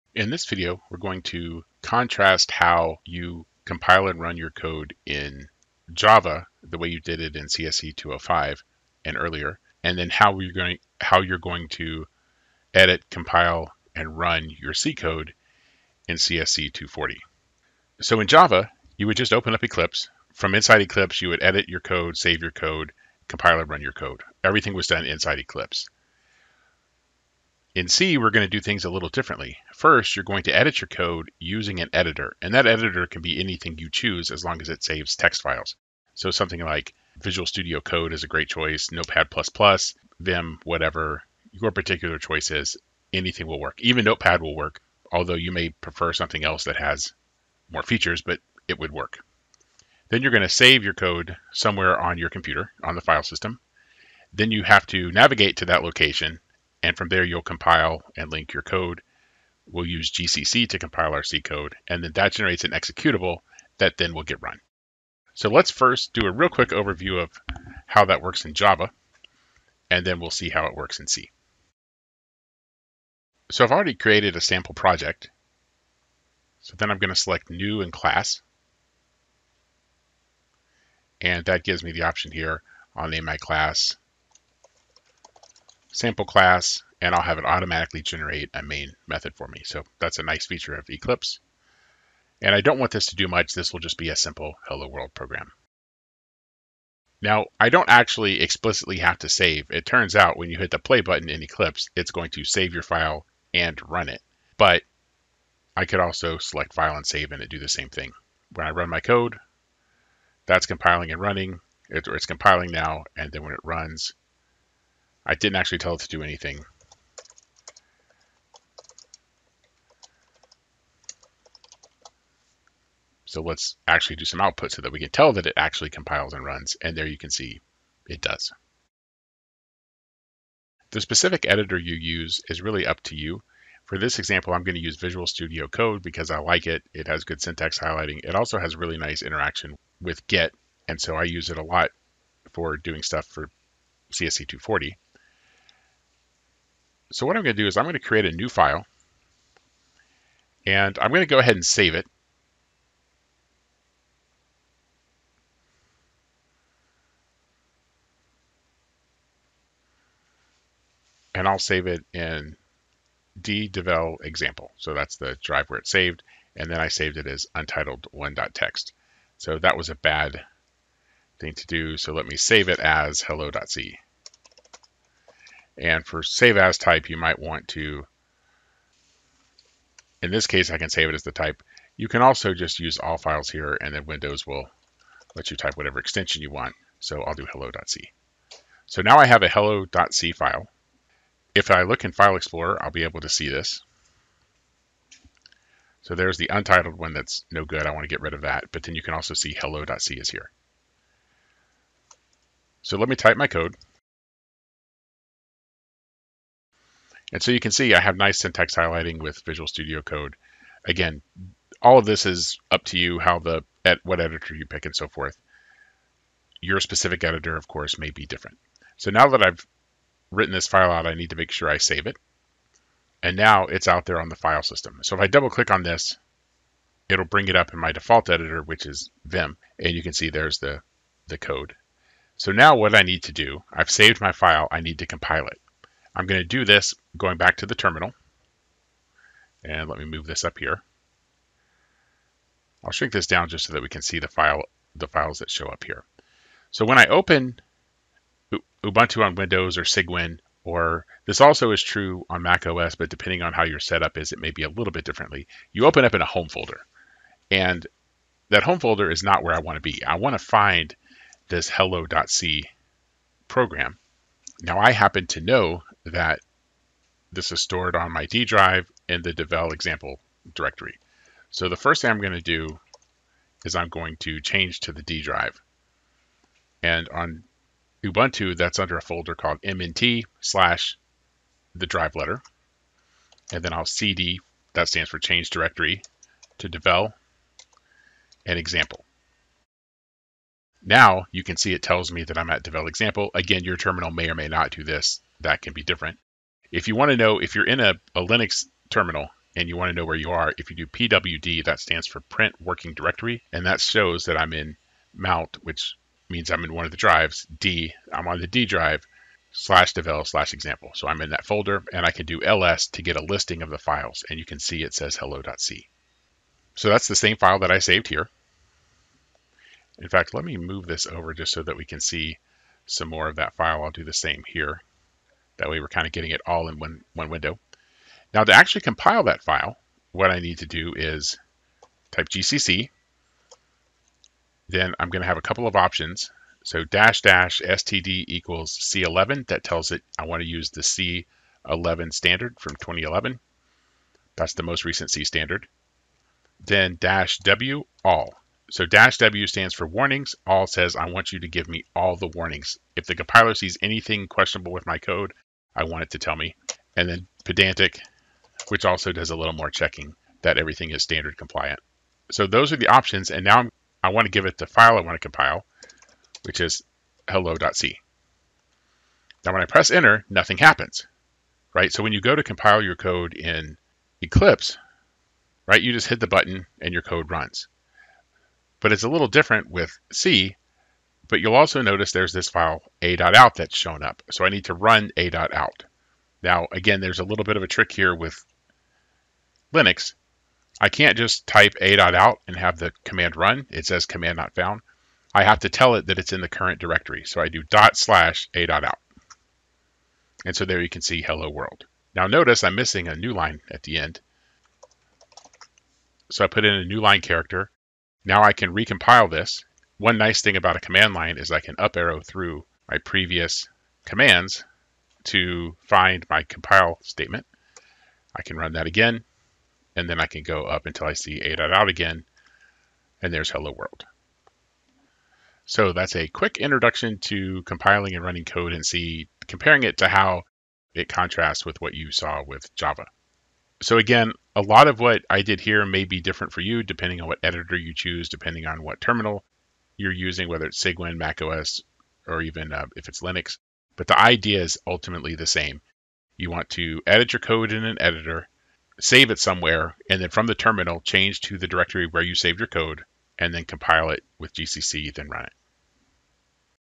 Introduction to Compiling C with gcc for Java Programmers
The intention of this video is to give Java programmers an introduction to compiling C code under Linux.  This example uses VS Code as the editor (though any text editor will work) and Ubuntu for Windows Subsystem for Linux.  Compiling in any other Linux distribution or MacOS follows a similar process.

0:00 - Overview of running Java code with Eclipse
0:27  - Overview of running C code with gcc (under Windows with WSL)
1:12 - Example of running Java code with Eclipse
2:11 - Editing C code with Windows (using VS Code)
3:11 - Using Windows Explorer to view the files
4:07 - Navigating to the code directory using the Ubuntu command line (bash)
5:28 - Compiling the code
6:20 - Executing the code (note that for Cygwin the executable will be a.exe instead of a.out)
7:05 - Review